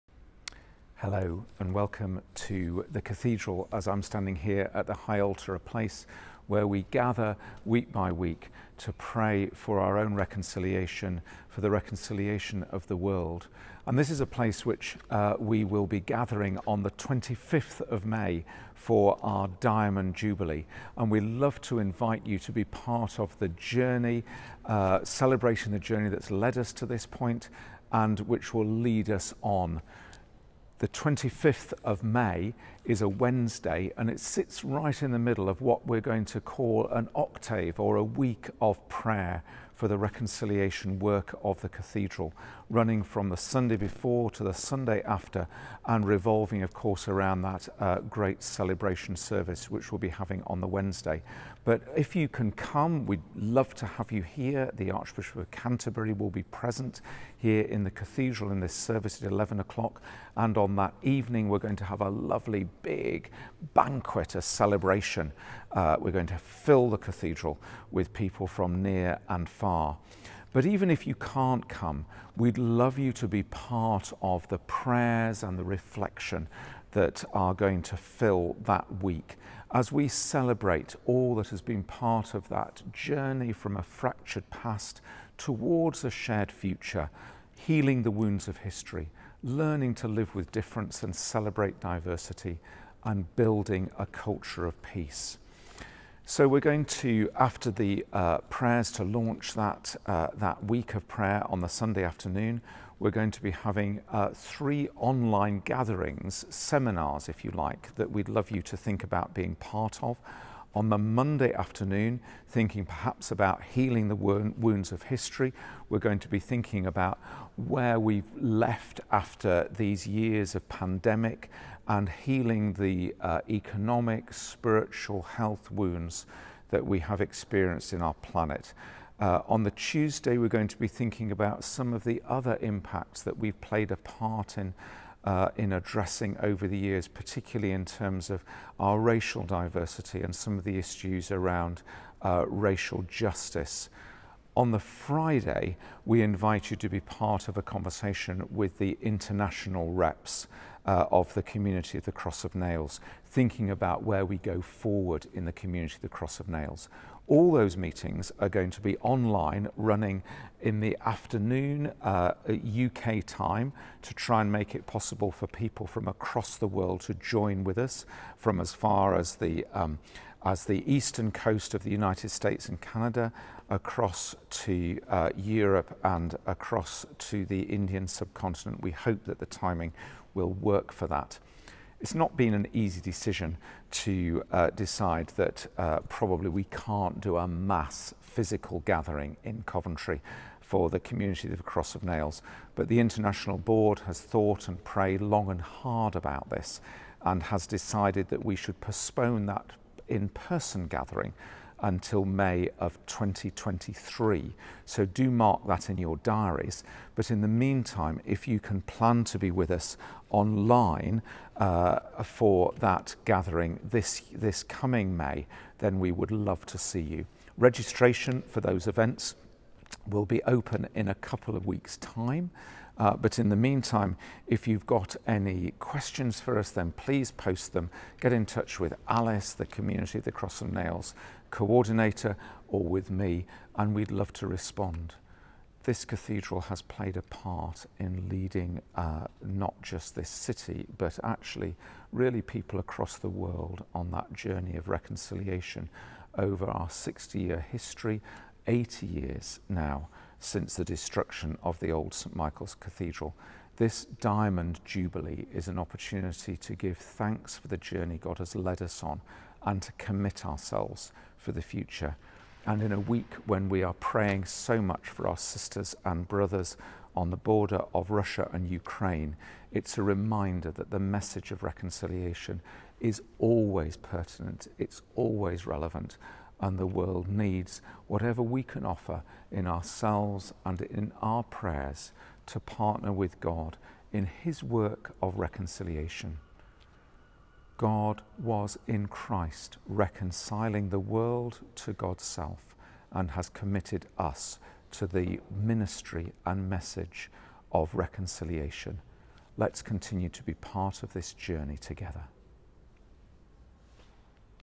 A prayer in a week when the world needs the power of our collective prayers
An invitation to mark the Cathedral’s 60th anniversary on and around 25 May, in a week when the world needs the power of our collective prayers.